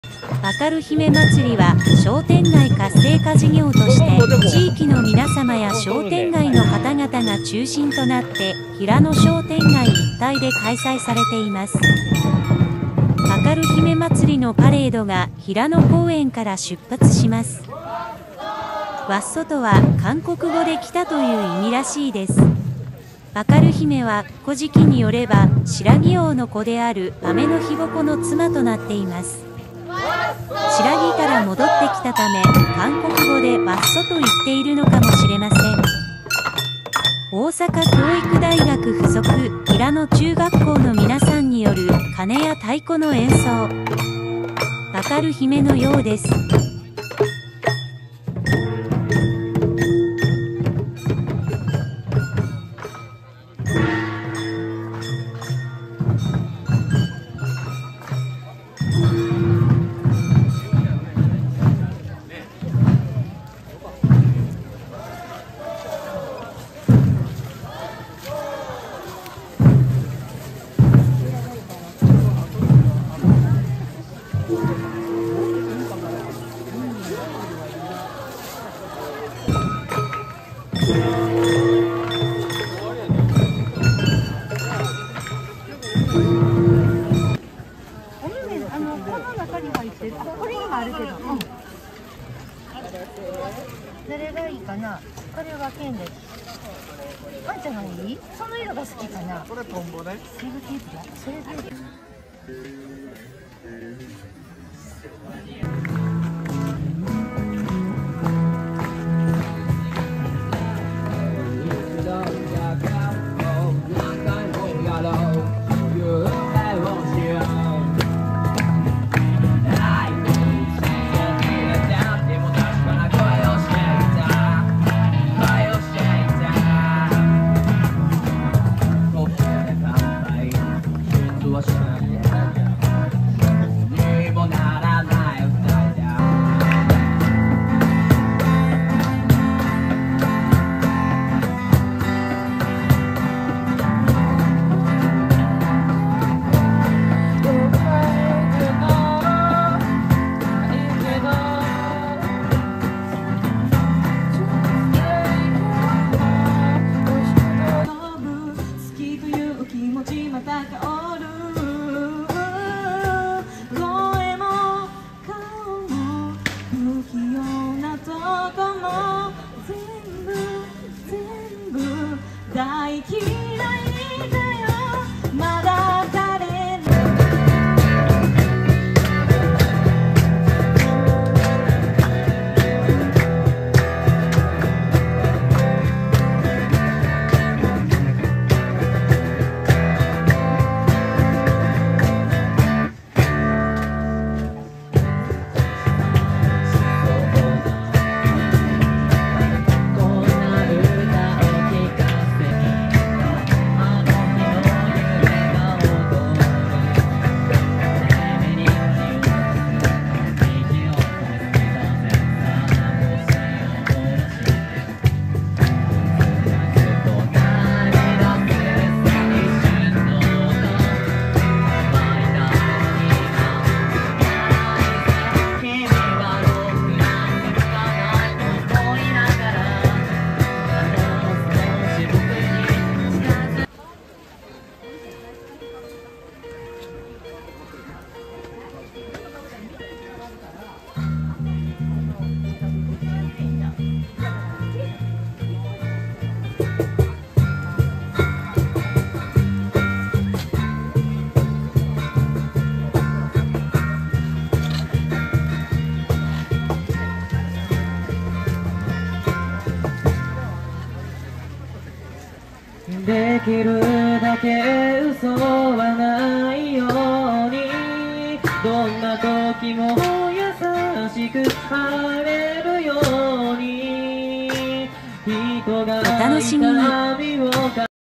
第１５回あかる姫まつり あかる姫まつりパレード 軽音楽部
今年三十歩神社と平野公園で行われたあかる姫まつりのイベントの様子です

あかる姫の行列が三十歩神社を出発して、平野公園を通って、平野商店街へ向かいます。
公園の広場では天王寺学館の生徒さんたちによる軽音楽部の演奏と歌がありました〜